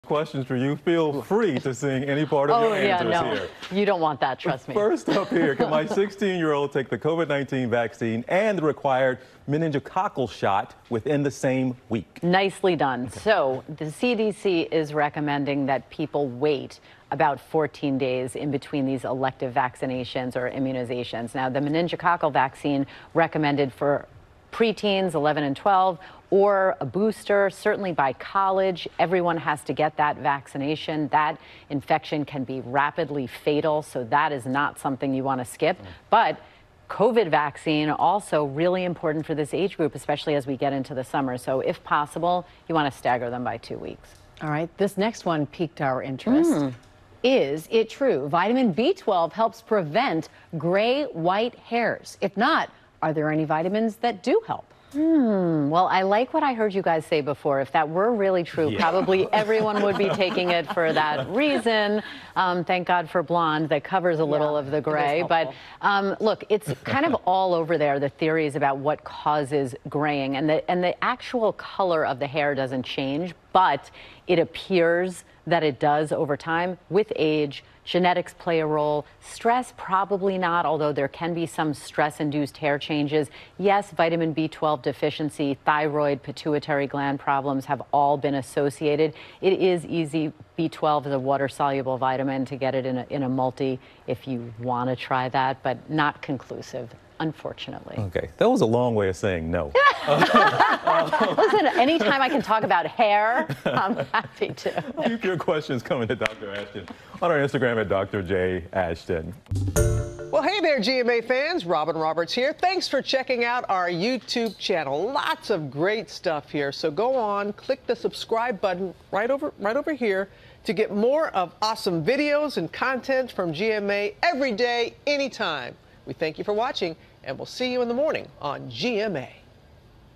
Are there vitamins that help prevent grey hairs?
Dr. Jen Ashton answers viewers’ questions about health, nutrition and COVID-19 vaccines.